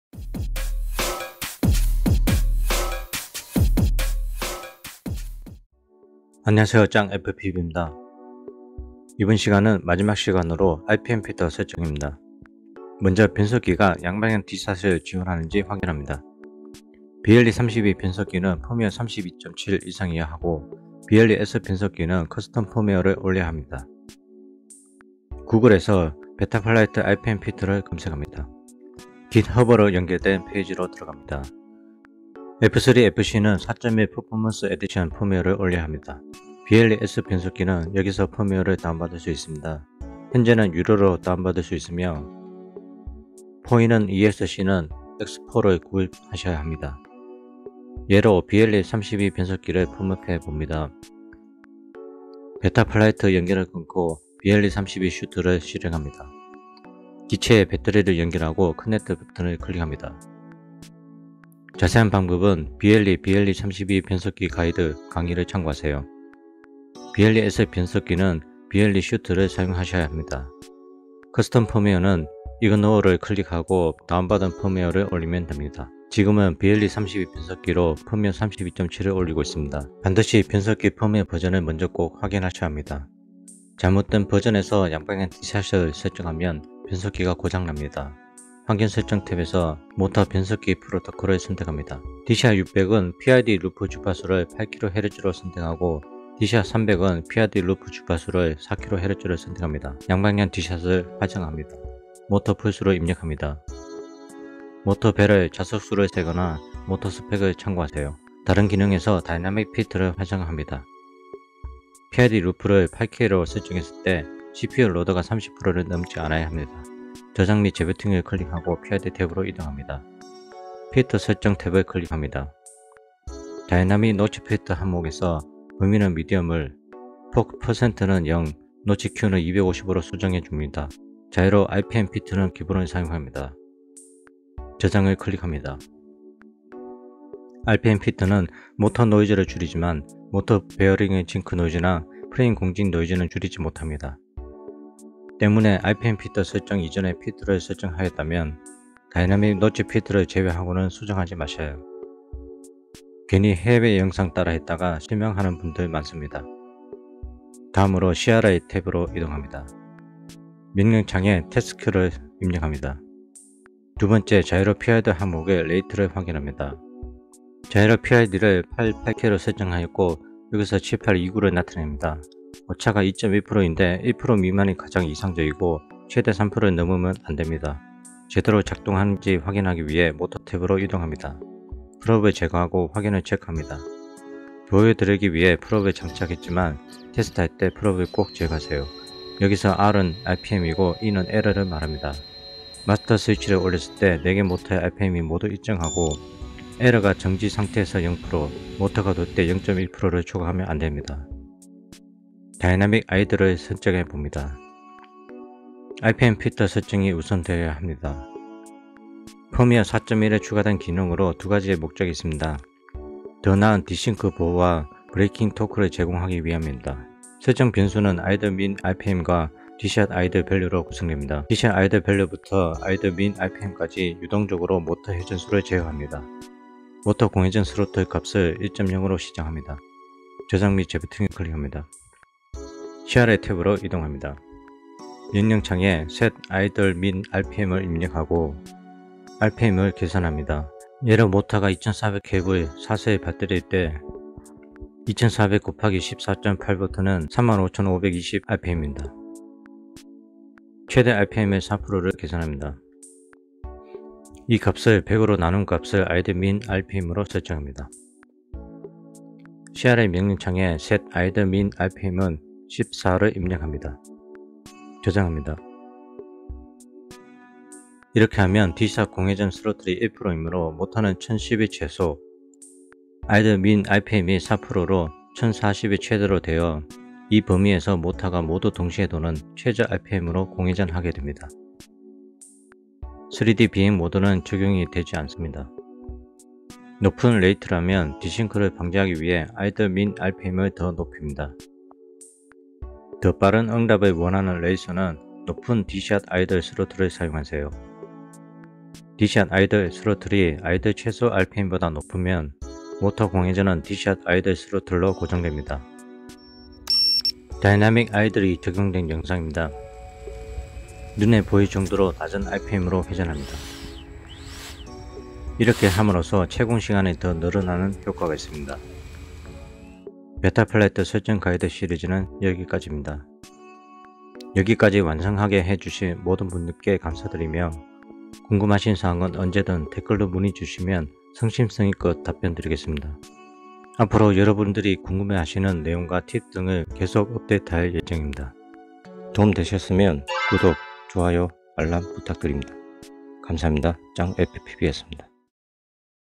[베타플라이트 설정 가이드] PID 3부-응용편(펌웨어 4.1.x) | RPM 필터, 다이나믹 Idle | Betaflight Set Up | 레이싱드론 세팅 | JJang FPV
베타플라이트 설정 가이드 시리즈
- 이번 시간은 PID 설정 3부입니다.(3/3)
* 펌웨어 4.1.x 에서 적용 가능합니다.

시리즈 마지막 편으로 RPM 필터와 다이나믹 Idle(공회전)을 설정하는 방법을 배워봅니다. 지금까지 강의들을 사랑해 주신 모든 분들께 감사드립니다. 베타플라이트 4.1 설정 가이드 시리즈는 여기서 마칩니다. 이후 더 좋은 컨텐츠로 찾아뵙겠습니다. 감사합니다.

[강의 목록]
* 아래 순서대로 보시는 걸 추천드립니다.